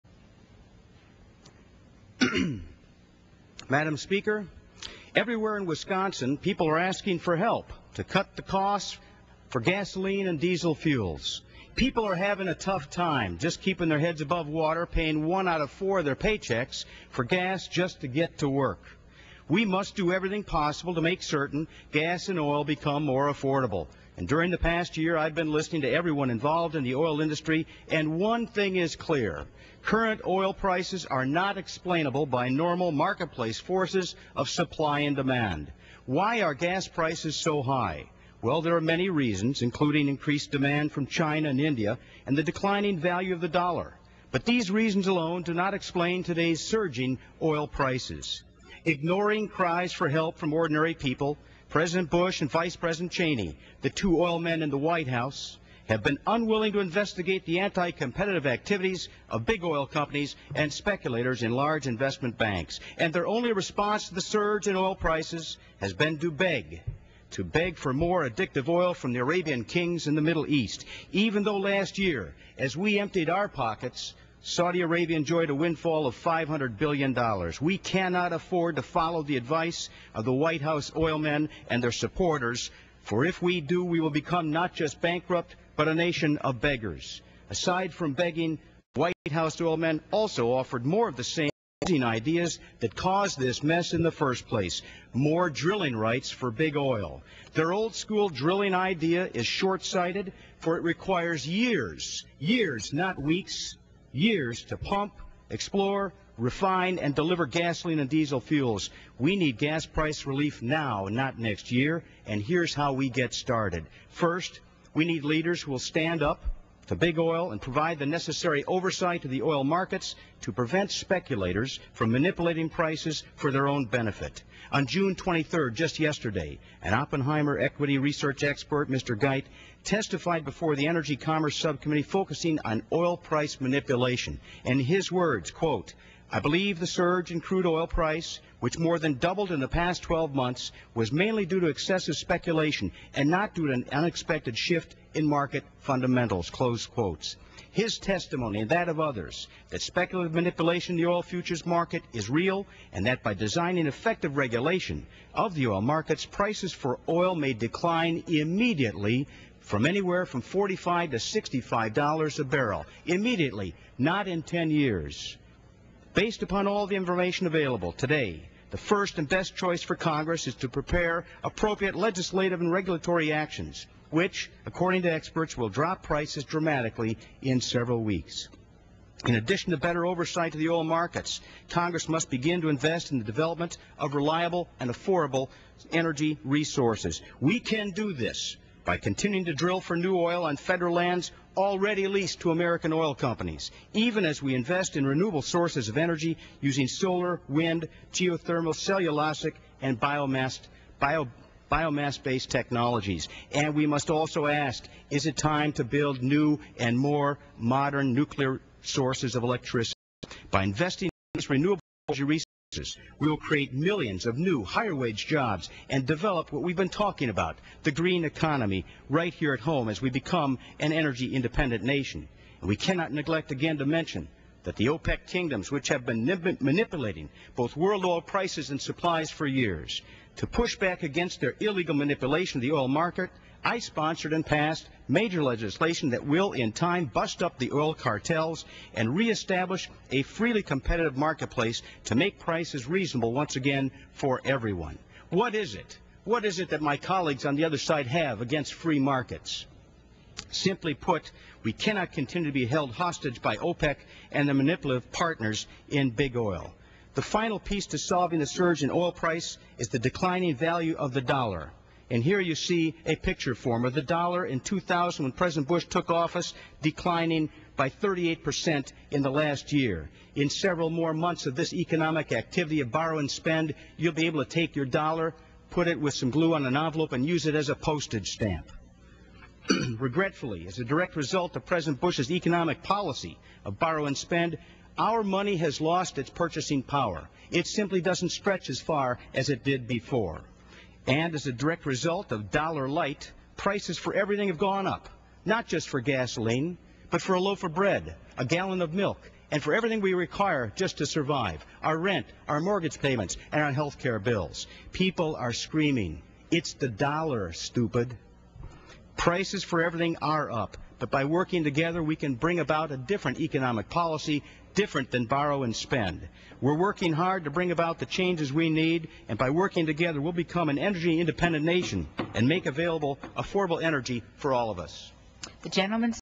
Working Together We Will Become an Energy Independent Nation
Congressman Steve Kagen, M.D. spoke on the House floor about our nation's energy crisis - short term and long term solutions.